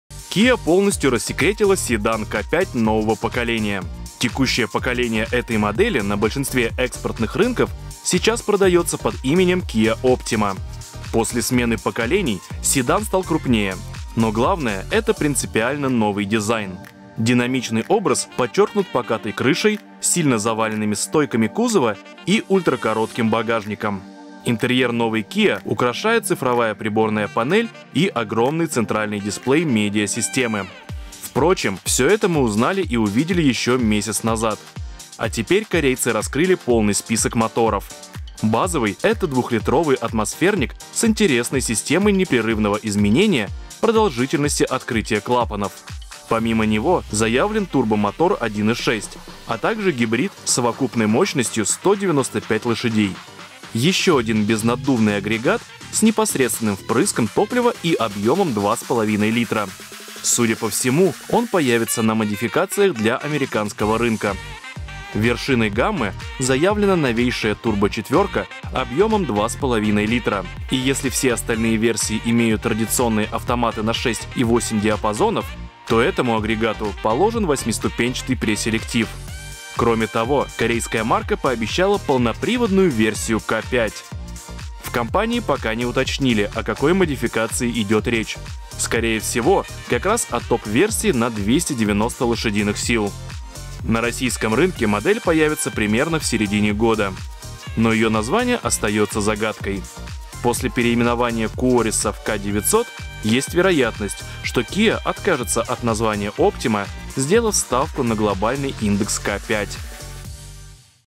Kia K5 примерит полный привод. А наша Киа Оптима? | Новости с колёс №678
Kia полностью рассекретила седан K5 нового поколения. Текущее поколение этой модели на большинстве экспортных рынков сейчас продаётся под именем Kia Optima.